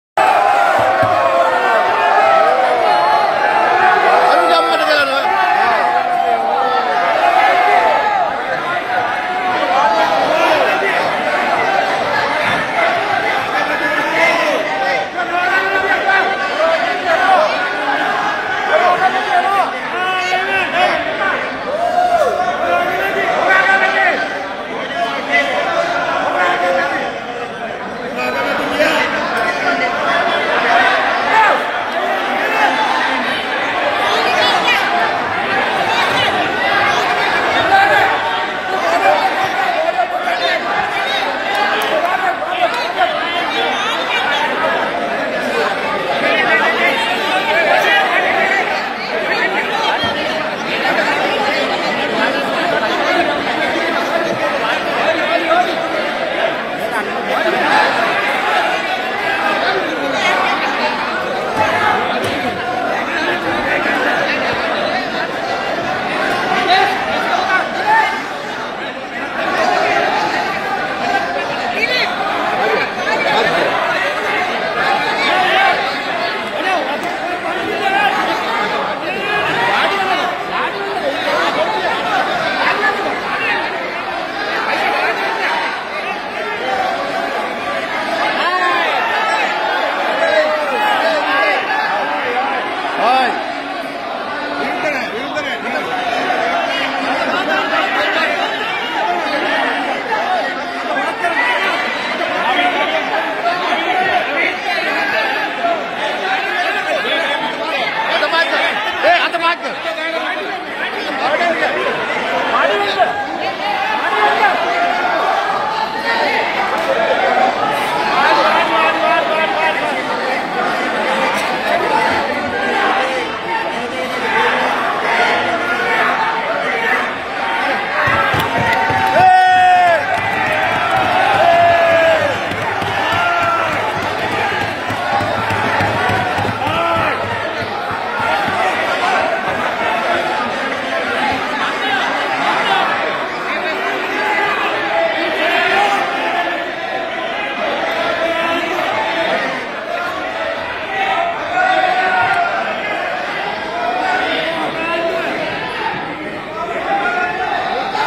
Prime Minister Mahinda Rajapaksha Walks out of Parliament
Footage of Prime Minister Mahinda Rajapaksha Walks out of Parliament